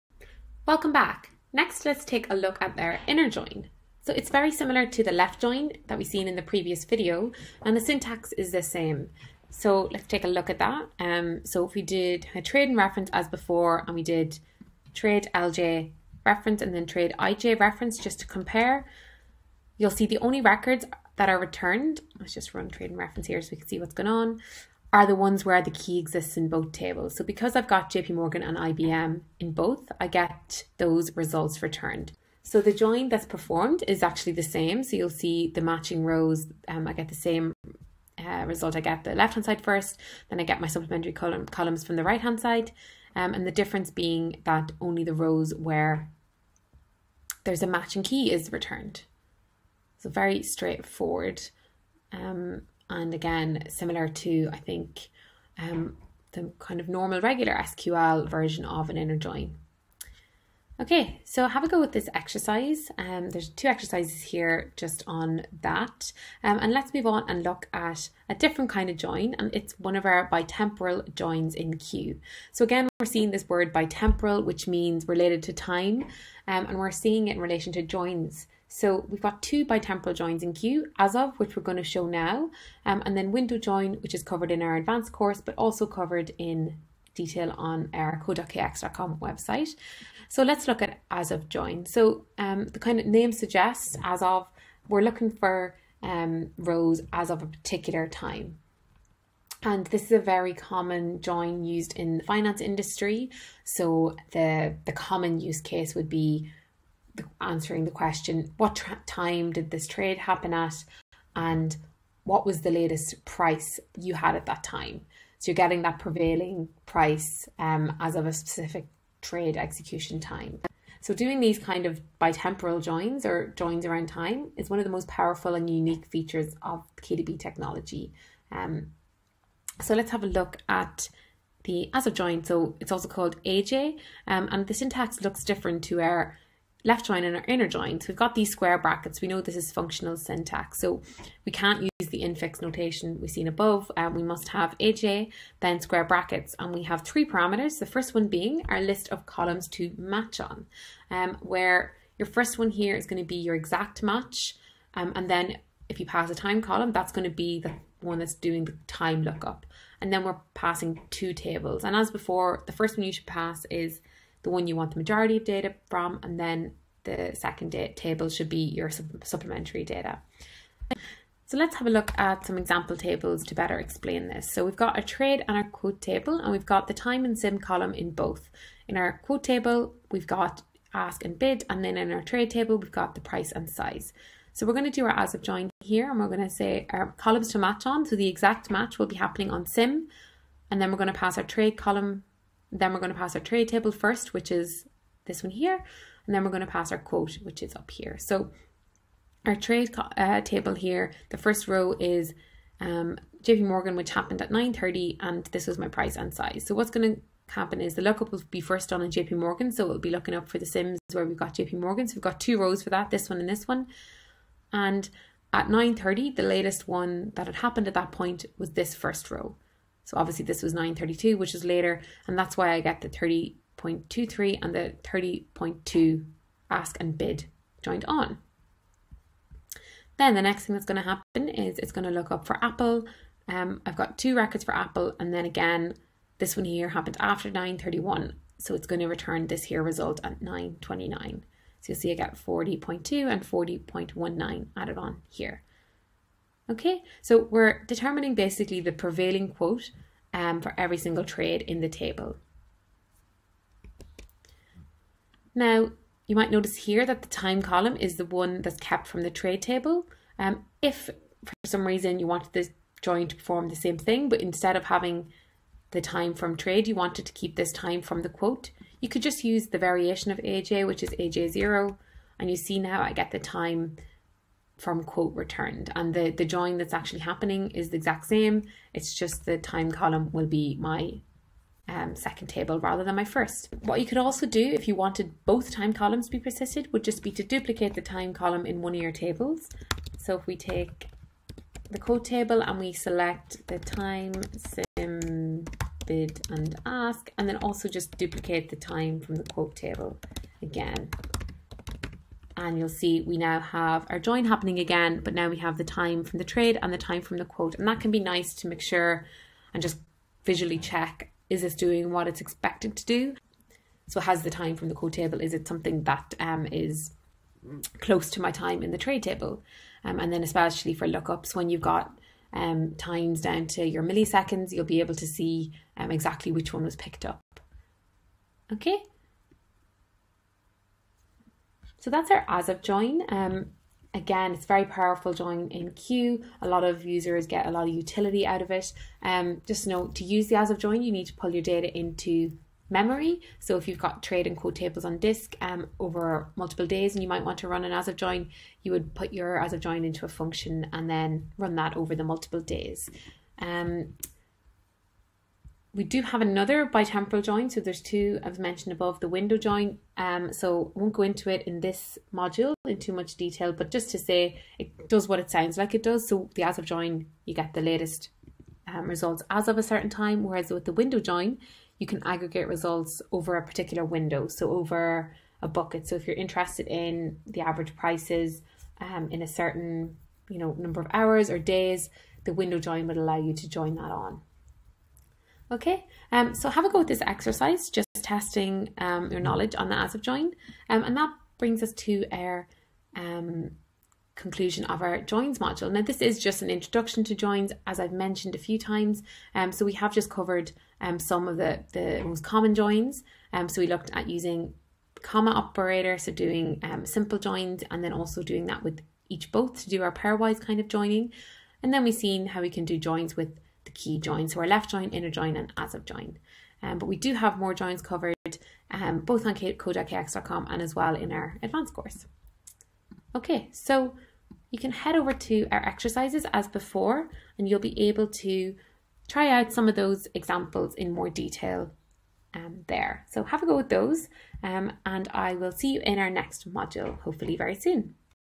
Fundamentals | Inner Join & asof Join in kdb | Hands on
The last video in the Table Joins module of the Fundamentals course looks at the asof join, a bitemporal join, and the inner join.

For the full KX Academy experience, including non-video Advanced Modules,